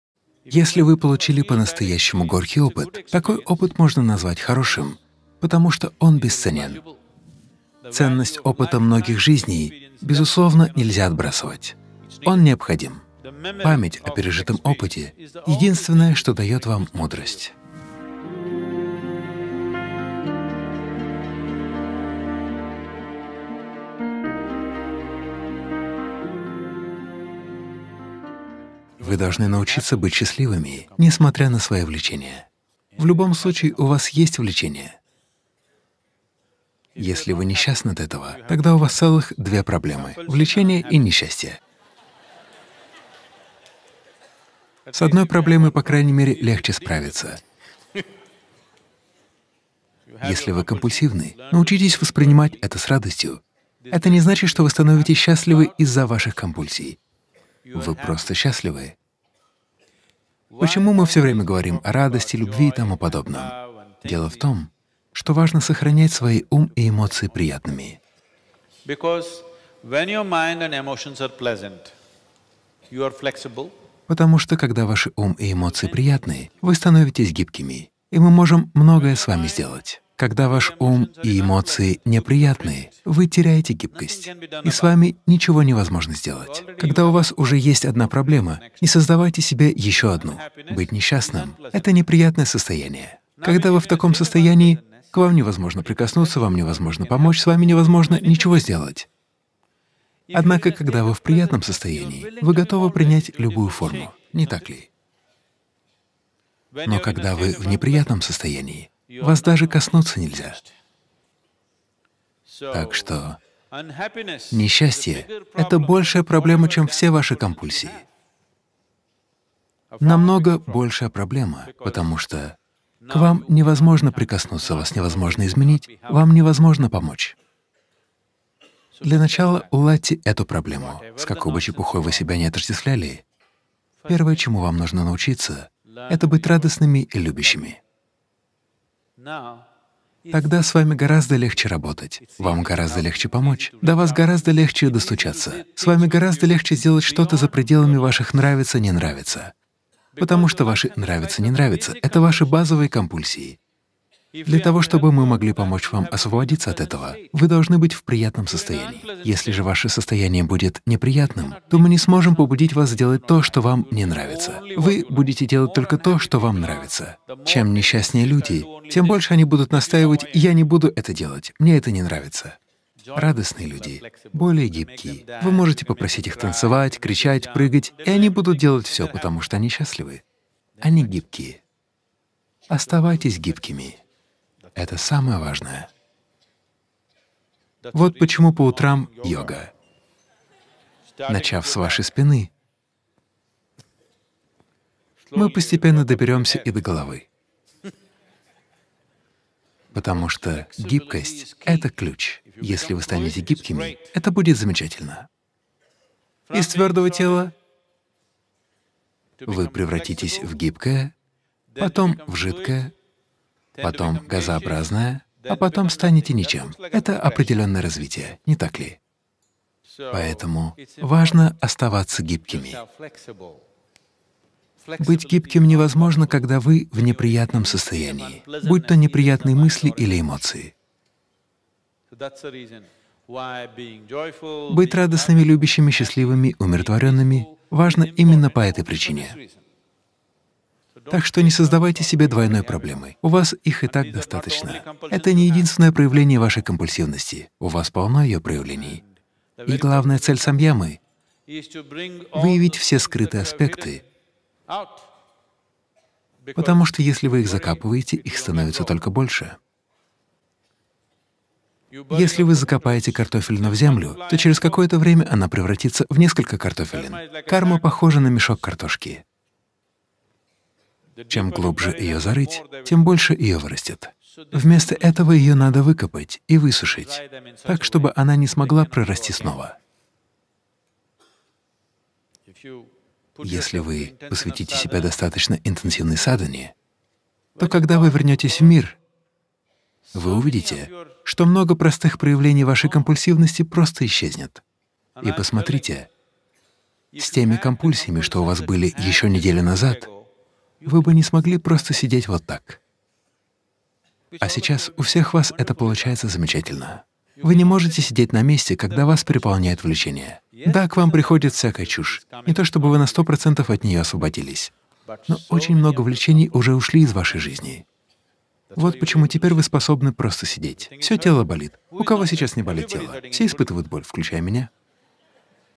Гибкость — ключ к полноценной жизни. Садхгуру
Многие люди страдая от последствий своих действий, винят в своих бедах судьбу или говорят о плохой карме. Но какое влияние оказывает на человека карма на самом деле? Используя метафоры и яркие образы, Садхгуру говорит о природе кармы и показывает, как стоит относиться к ней.

Садхгуру: Вы должны научиться быть счастливыми, несмотря на свою компульсивность. В любом случае, она у вас есть. Если вы еще и несчастны от этого, тогда у вас целых две проблемы: компульсии и несчастье. Когда у вас уже есть одна проблема, не создавайте себе еще другую. По крайней мере, с одной проблемой легче справиться.

Быть несчастным — это неприятное состояние. Мы всегда говорим о радости и любви для того, чтобы ваши ум и эмоции оставались в приятном состоянии. Когда ваши ум и эмоции приятны, вы становитесь гибкими, и мы можем многое с вами сделать. Когда ваш ум и ваши эмоции неприятны, вы теряете гибкость, и с вами ничего невозможно сделать. Если вы пребываете в приятном состоянии, то готовы принять любую форму. Когда вы находитесь в неприятном состоянии — вас даже коснуться нельзя.

Состояние несчастья — это более серьезная проблема, чем любая компульсивность!
Это гораздо более серьезная проблема потому, что тогда нельзя ни прикоснуться к вам, ни изменить вас, ни помочь вам. Если вы компульсивны, научитесь воспринимать это с радостью. Тогда с вами будет легко работать, легко вам помочь, легко достучаться до вас и сделать с вами что-то за пределами ваших симпатий и антипатий. Ваши симпатии и антипатии — самые базовые проявления компульсивности. Для того чтобы мы могли помочь вам освободиться от этого, вы должны быть в приятном состоянии. Если же ваше состояние будет неприятным, то мы не сможем побудить вас сделать то, что вам не нравится, вы будете делать только то, что вам нравится. Чем несчастнее люди, тем больше они будут настаивать: «Я не буду это делать, мне это не нравится!» Радостные люди более гибкие — вы можете попросить их танцевать, кричать, прыгать, делать любые смешные вещи, например, ползать по земле, и они будут делать всё — потому что они счастливы. Они более гибкие.
Не теряйте свою гибкость. Это крайне важно. Когда вы в неприятном состоянии, под влиянием неприятных мыслей или эмоций, быть гибким невозможно. Поэтому быть радостными, любящими, счастливыми, умиротворенными важно хотя бы из-за того, что в вас много компульсивности.
Если вы умны, вы всегда будете помнить свои неприятные переживания без обиды или гнева — так, чтобы никогда больше не оказаться в подобной ситуации.
Карма похожа на мешок картошки. Чем глубже вы "зарываете" свою кармическую "картошку", то есть накопленные навязчивые склонности, — тем больше их вырастет. Садхана заключается в том, чтобы "доставать" и "высушивать" эту "картошку" до такой степени, чтобы она не смогла "прорасти" снова. Если вы посвятите себя достаточно интенсивной садхане, то, когда вы вернётесь в мир, вы увидите, что много простых проявлений вашей компульсивности просто исчезнет. Но вам предстоит "выкопать" еще много кармической "картошки". Нужно ежедневно "доставать и высушивать" её до такой степени, чтобы она не смогла "прорасти" снова.
Если одна и та же кармическая "картофелина" прорастёт снова, то из нее получится еще много новых "картофелин". Допустим, у вас было 5 минут интенсивного переживания боли или страха. Всего 5 минут болезненного или отвратительного опыта. Но эти 5 минут могут разрастись так, что определят всю вашу жизнь. Куда бы вы ни пошли, вы будете говорить и думать только об этом. Вы закопали свою кармическую "картошку" — и её стало ещё больше. Одна "картофелина" превратится в миллион таких же отвратительных "картофелин". Теперь же "высушите" их и сложите отдельно — пусть будет, это отличный опыт. Даже если вы пережили действительно болезненный опыт, это всё равно станет отличным опытом. То, чему вы научились в неприятные моменты вашей жизни, — бесценно!
С какой бы чепухой вы себя не отождествляли, первое, чему вам нужно научиться, — это быть радостными и любящими.
Мы не стремимся избавляться от нашего "картофеля" кармы, просто хотим "выкопать и высушить" его так, чтобы он не "пророс". Мы хотим сохранить его, иначе так и останемся невежественными глупцами, которые снова и снова наступают на те же грабли. Цените свой жизненный опыт! Не стоит недооценивать важность жизненного опыта. Но если вы позволите ему прорасти в вас, он отравит всю вашу жизнь.

Карма, или память о пережитом опыте, — это единственное, что даёт вам мудрость. Но также это единственное, что может связать вас, единственное, что может настолько глубоко отравить вашу жизнь, что это разрушит вас. Поэтому карма — это не проблема, проблема в том, как вы с ней обращаетесь